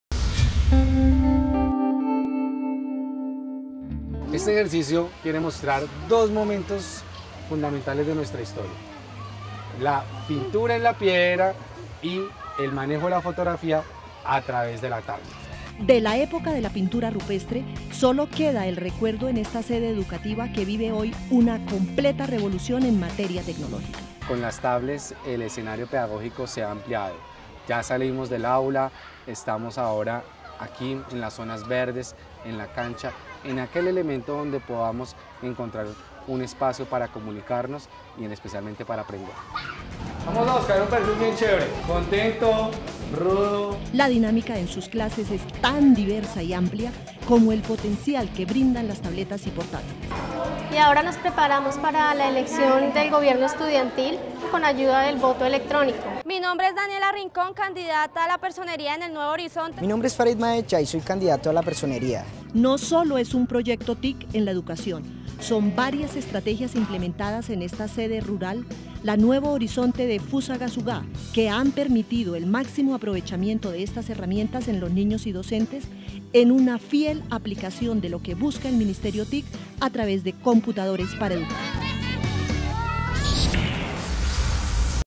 Con las tabletas el escenario pedagógico se transforma
Los proyectos y estrategias pedagógicas implementadas en las instituciones educativas, permiten que la apropiación de la tecnología genera nuevas formas de aprendizaje